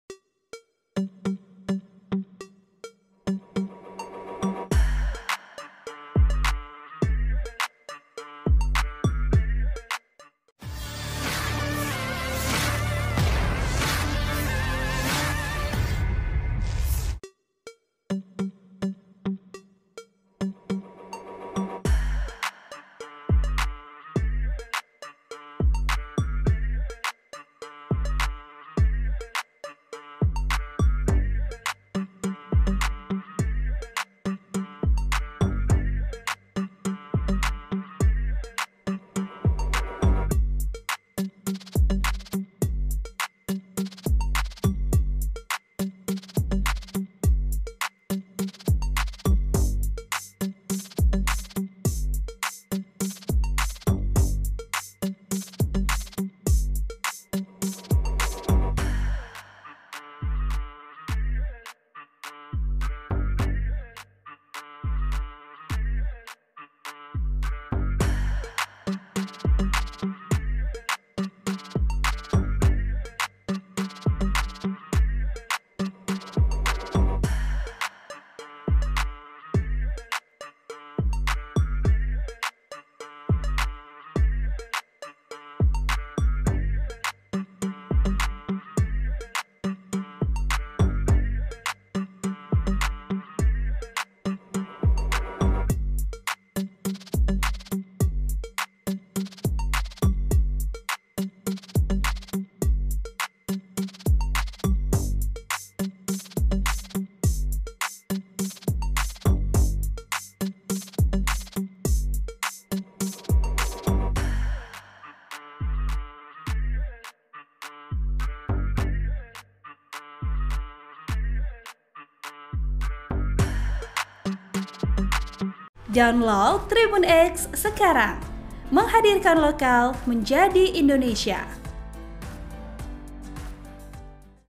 Aksi Kerja Tempur Tentara Ukraina Bombardir Konvoi Tank Rusia saat Melintas
Naskah: Nurul Arifah s Torro

(TRIBUN-TIMUR.COM)